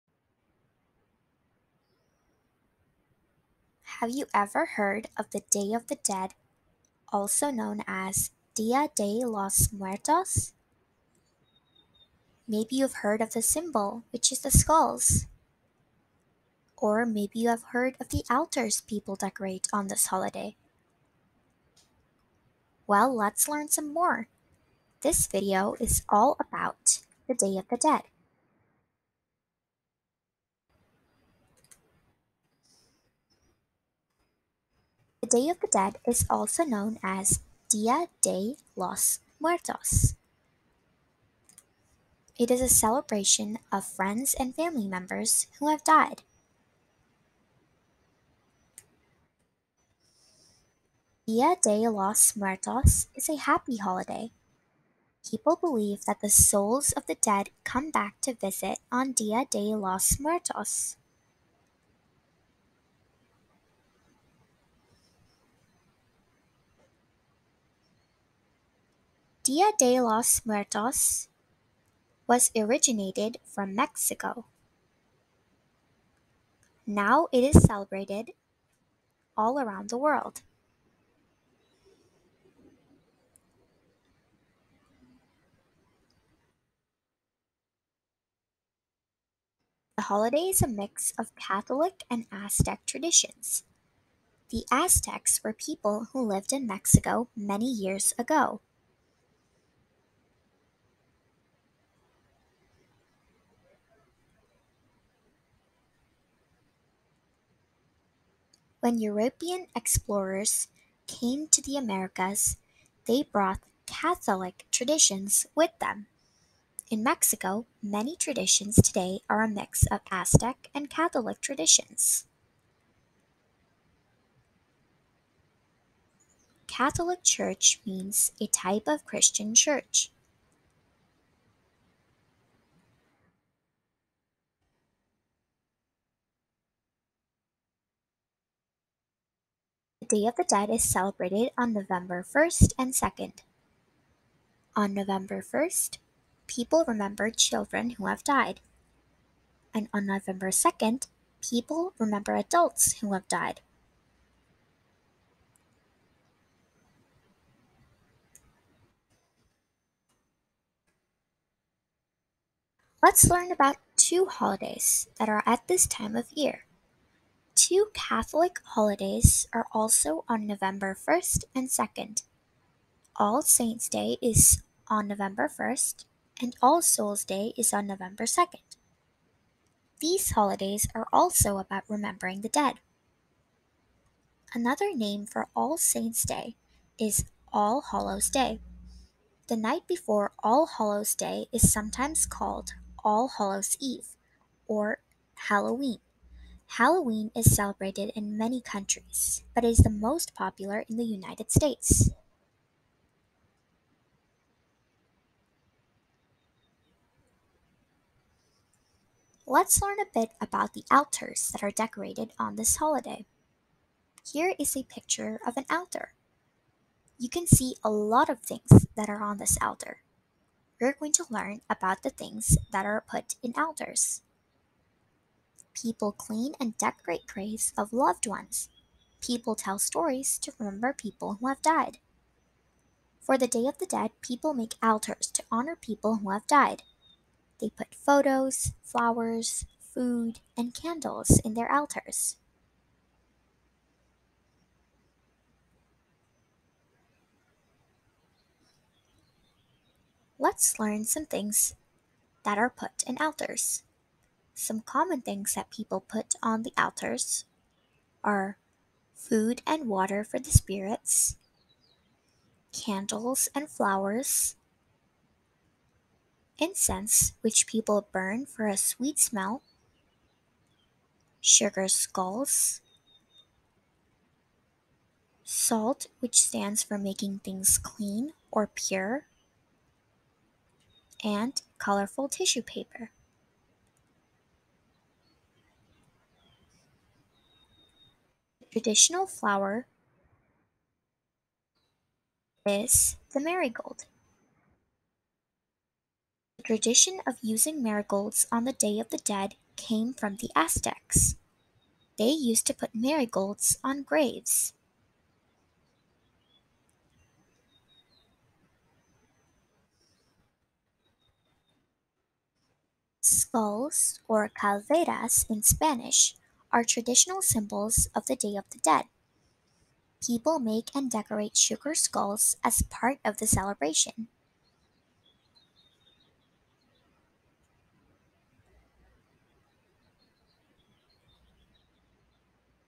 What is the Day of the Dead? || Interesting Facts about the day of the day/ basic GK for kids
Hey guys!

Let’s learn some AMAZONG facts about Dia de Los Muertos! It is also known as the day of the dead!